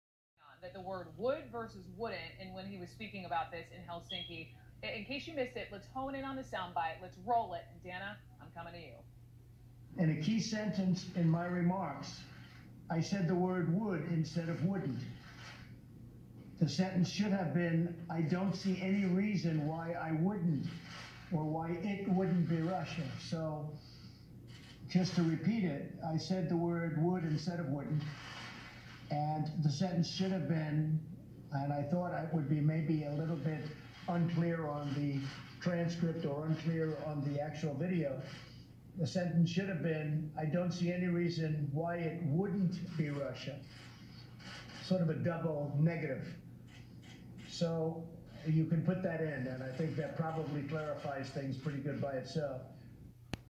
trump Would vs Wouldnt
This guy!!! Just when you think trump can't make things worse for himself. 🤯